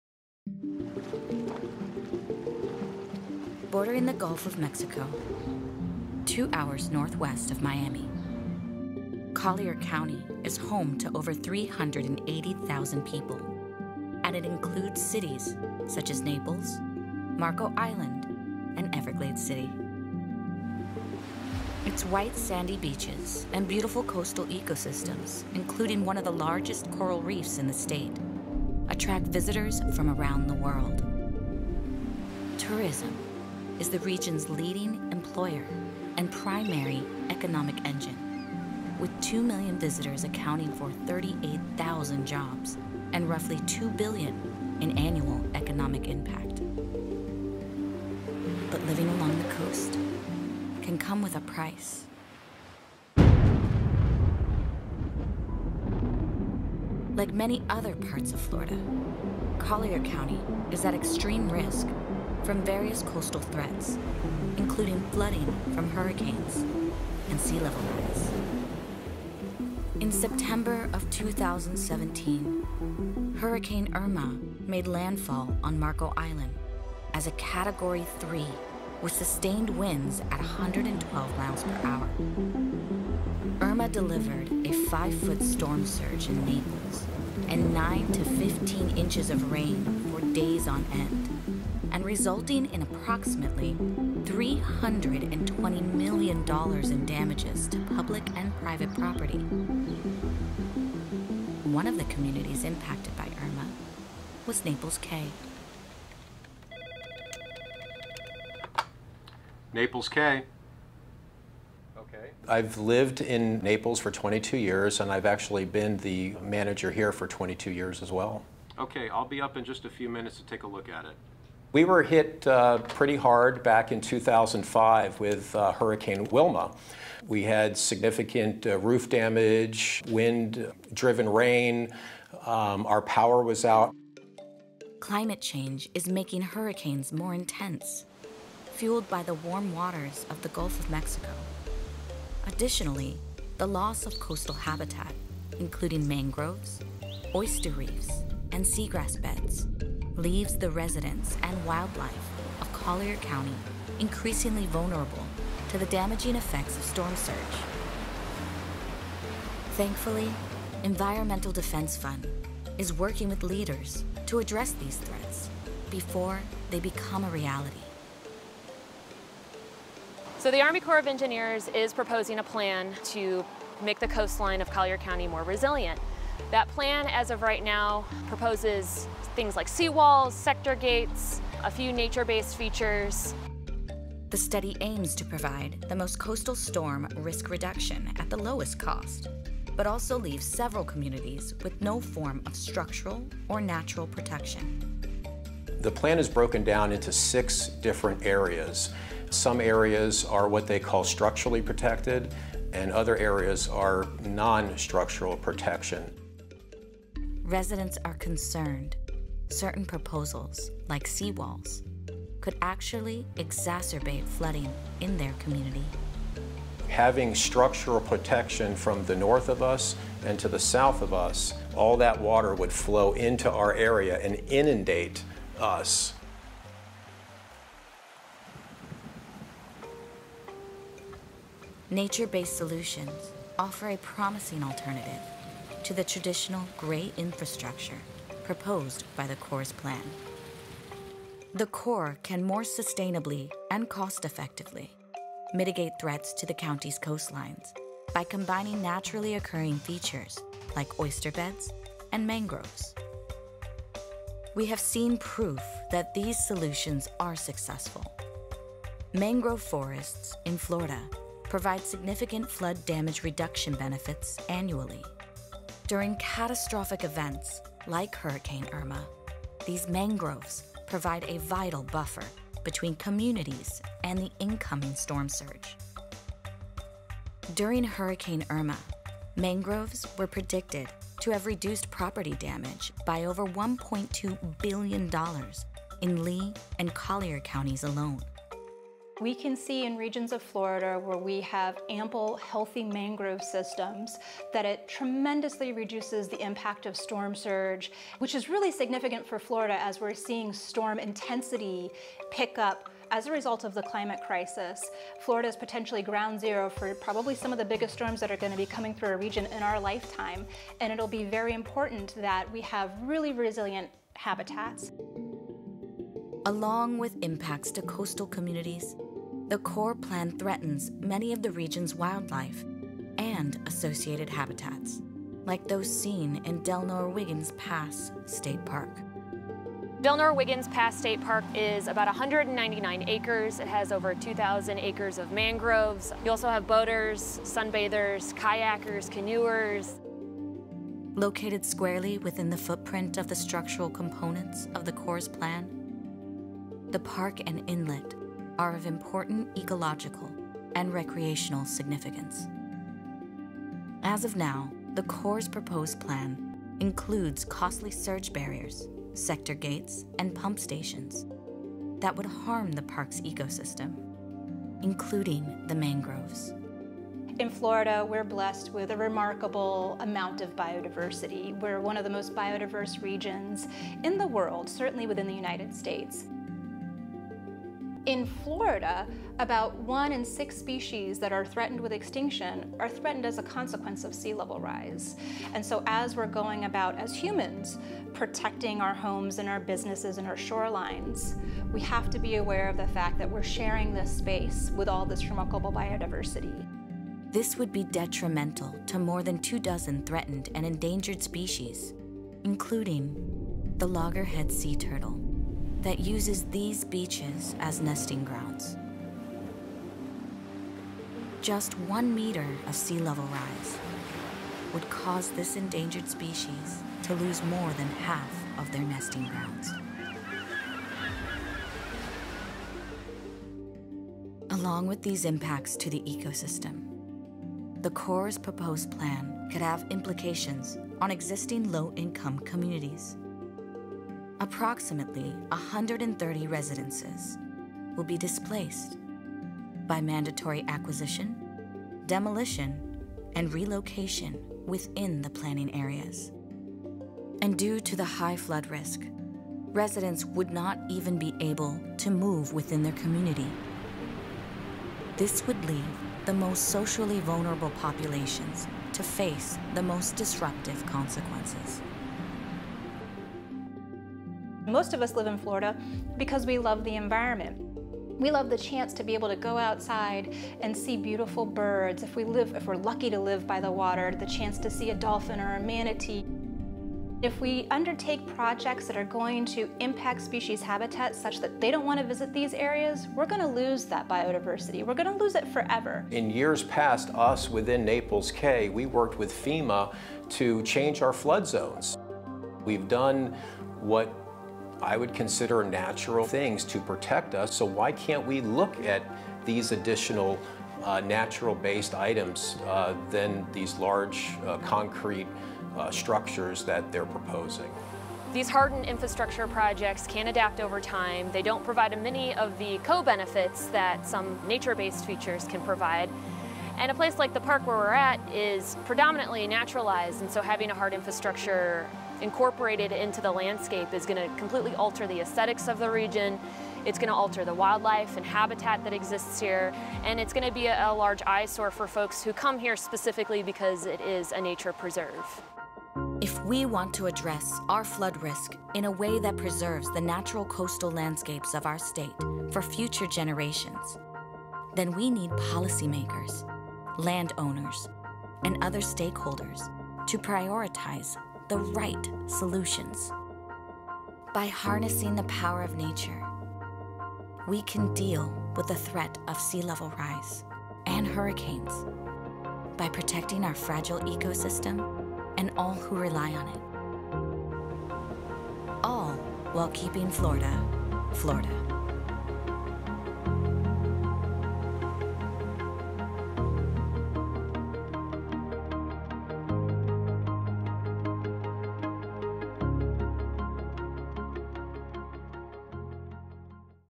Keeping Florida, Florida: Nature provides climate resilience and vital habitat in Collier County
Keeping Florida, Florida: Collier County, Florida is home to over 380,000 people in cities like Naples, Marco Island and Everglades City. In the face of climate change, the region is vulnerable to heavy precipitation, sea level rise and more intense hurricanes.

The U.S. Army Corps of Engineers has long favored structural solutions, like sea walls, surge barriers and flood gates, which create inconsistent flood protection and damage to Florida's vibrant coastal habitats.

As an alternative, natural infrastructure and hybridized designs that integrate features like mangroves and oyster reefs provide a cost-effective, multi-beneficial alternative that can increase climate resilience while preserving Southwest Florida's natural beauty.